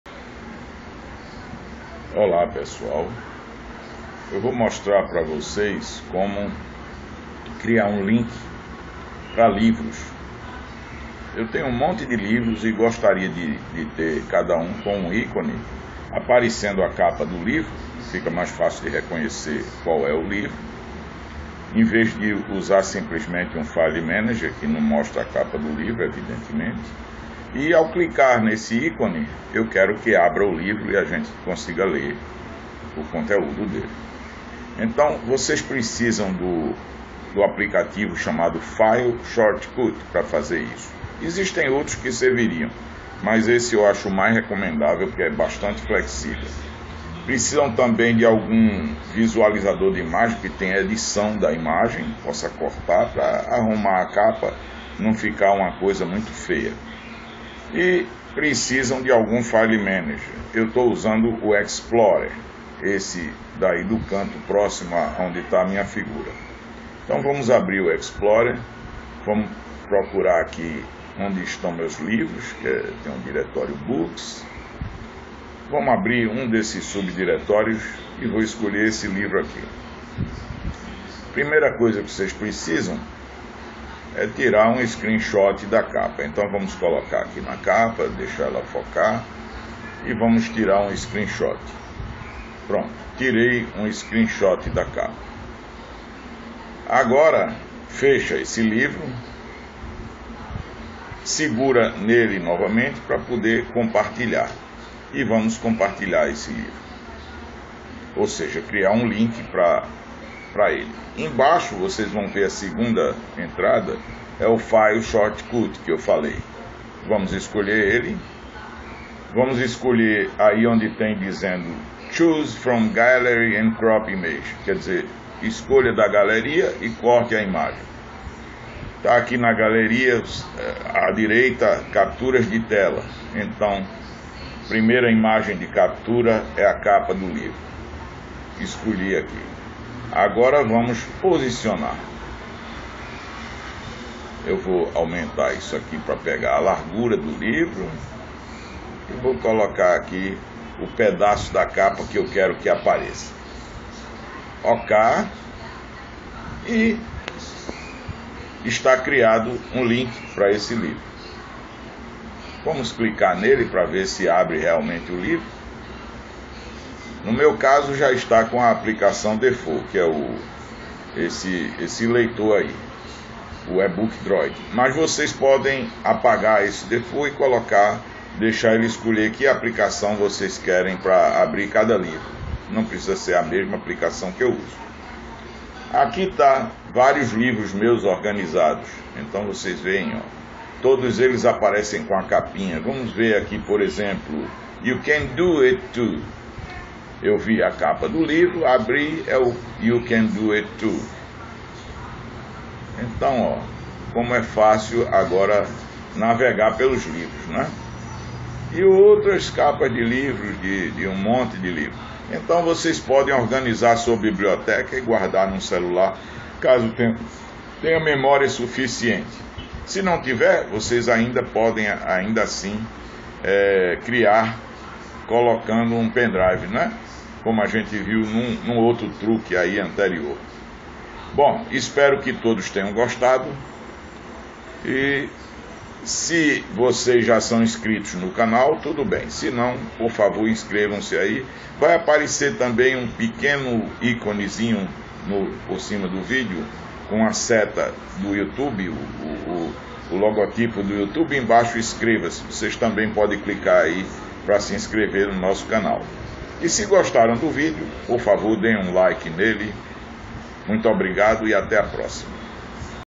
Como criar um ícone para um livro.
Você gostaria de ter seus livros no Android (celular ou tablet) na forma de ícones (icons) com as capas de cada livro aparecendo, de forma que clicando no ícone o livro correspondente é aberto?  Siga o roteiro mostrado aqui e poderá organizar seus livros em pastas por assunto ou da forma que achar melhor.